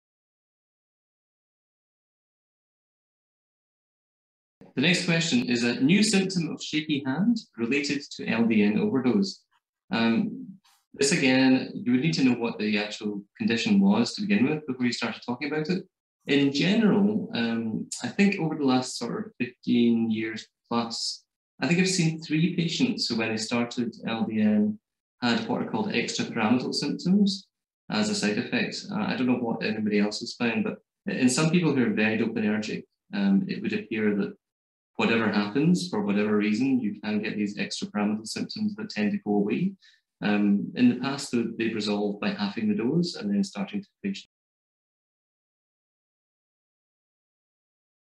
Shaky hands and Low Dose Naltrexone (LDN)
LDN Low Dose Naltrexone and shaky hands

the next question is a new symptom of shaky hand related to lbn overdose um this again you would need to know what the actual condition was to begin with before you started talking about it in general um i think over the last or 15 years plus i think i've seen three patients who when they started ldn had what are called extrapyramidal symptoms as a side effect i don't know what anybody else has found but and some people who are very open energy and it would appear that whatever happens for whatever reason you can get these extra parameter symptoms that tend to go away um in the past they've resolved by having the dose and then starting to finish
Answered by LDN Specialist Pharmacist Stephen Dickson